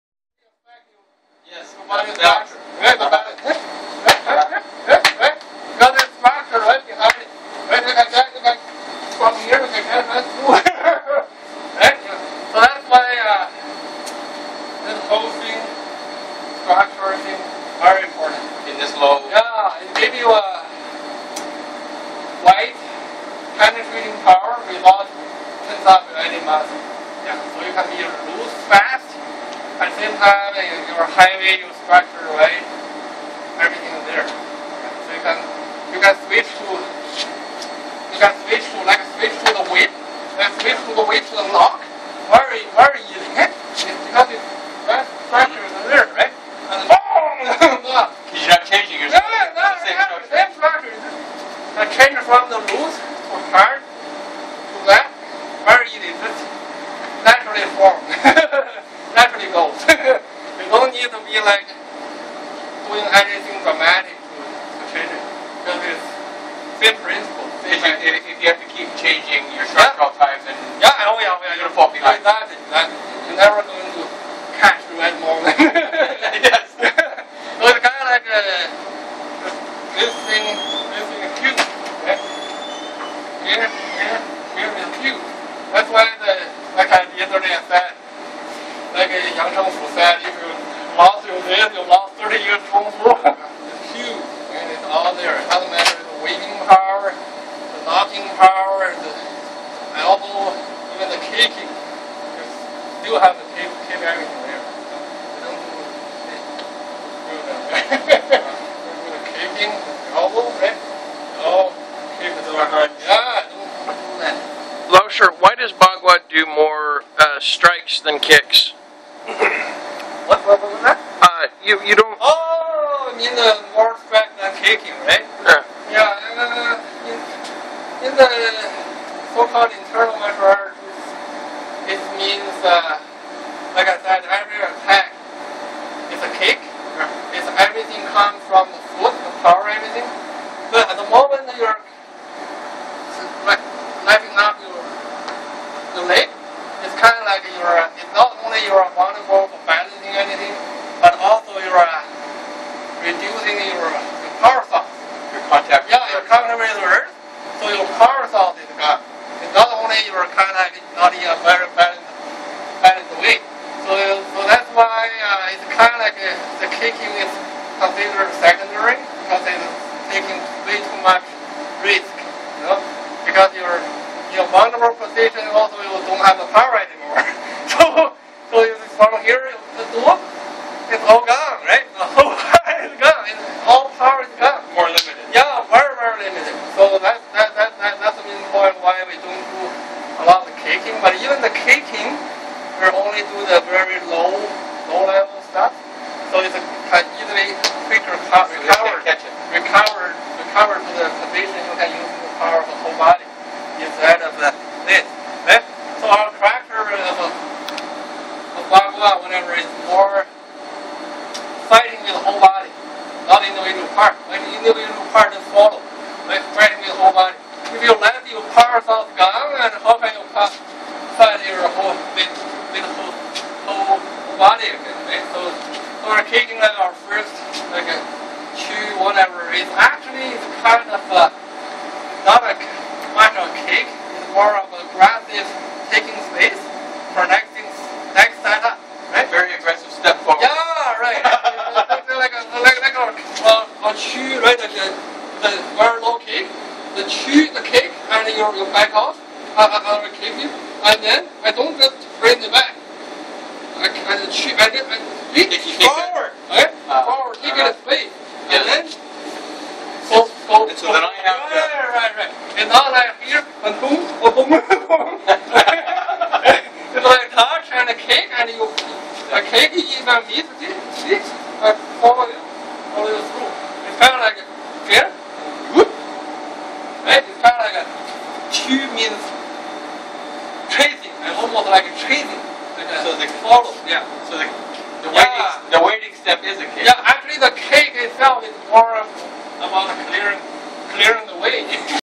Discussion about Gao Bagua's structure and kicking
Hey!  Just wanted to share some recent video I took of my teacher when I went to visit him.  Here he is talking about proper structure in the beginning and moves to a question about Bagua's kicking methods.